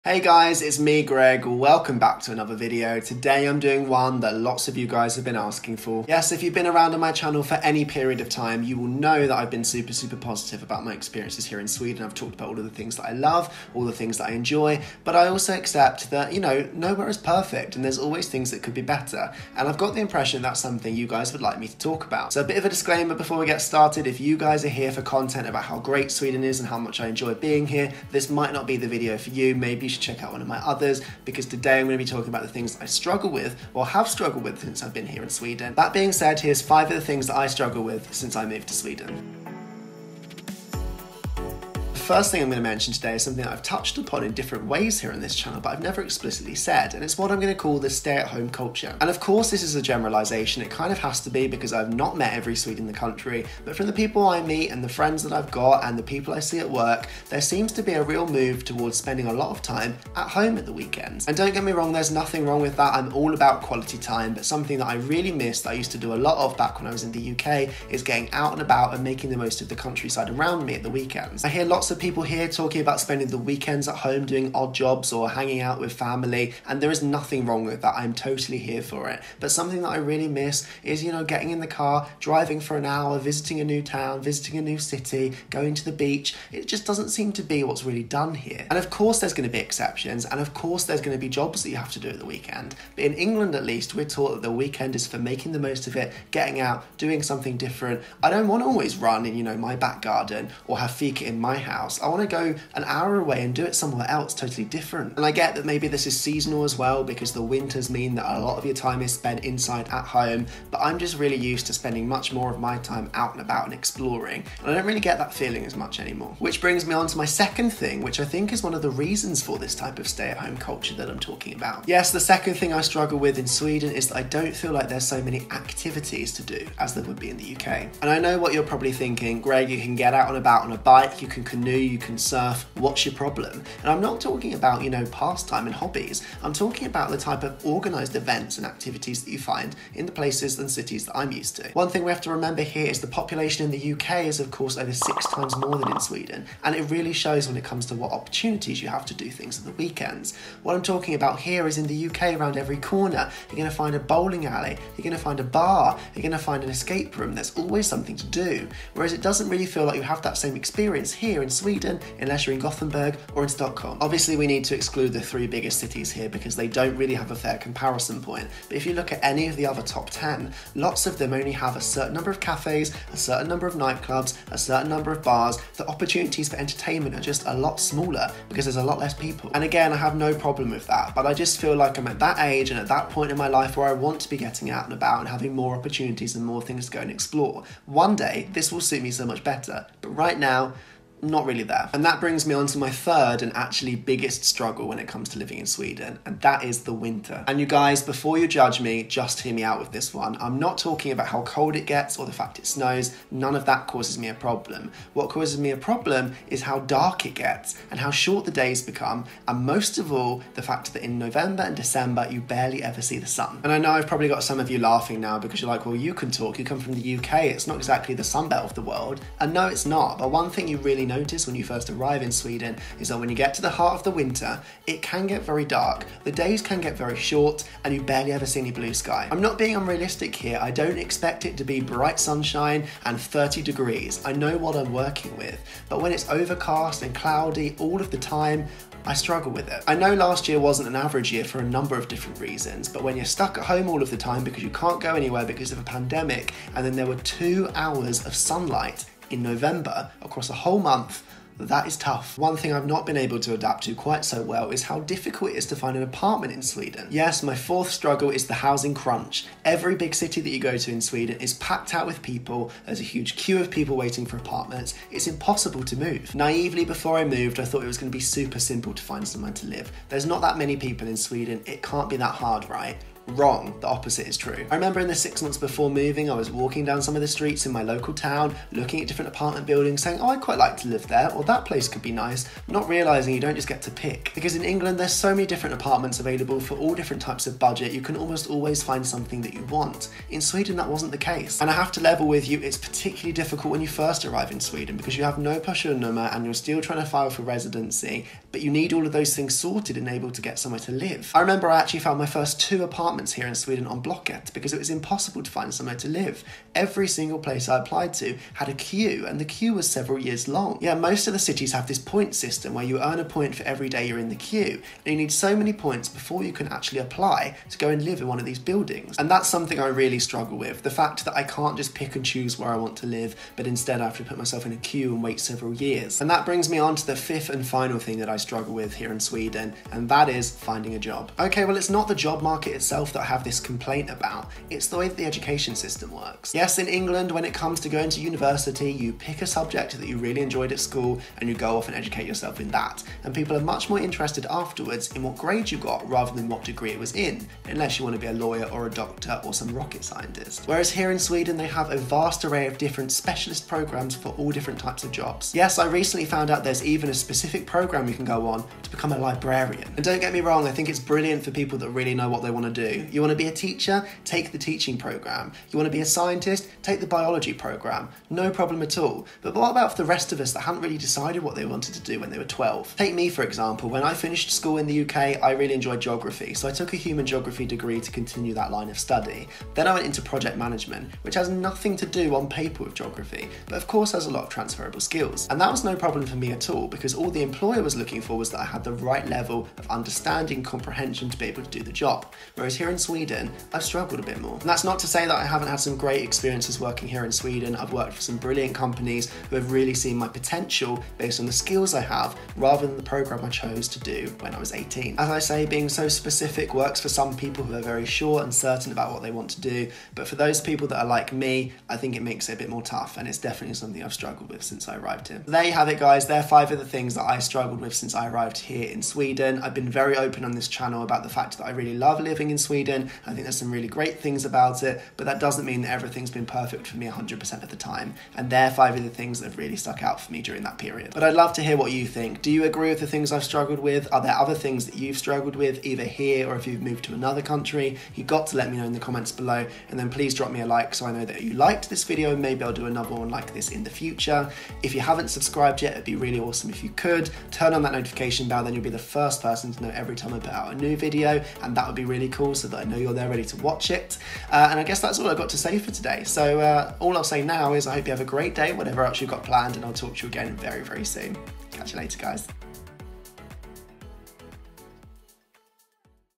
5 Things I’ve Found Hard Since Moving to Sweden - Just a Brit Abroad
What is it really like to live in Sweden? And what is the hardest part about moving there? What do I like the least about my life here? In this video, I count down the things I find difficult since making the move to Sweden and the things I struggle with about living here.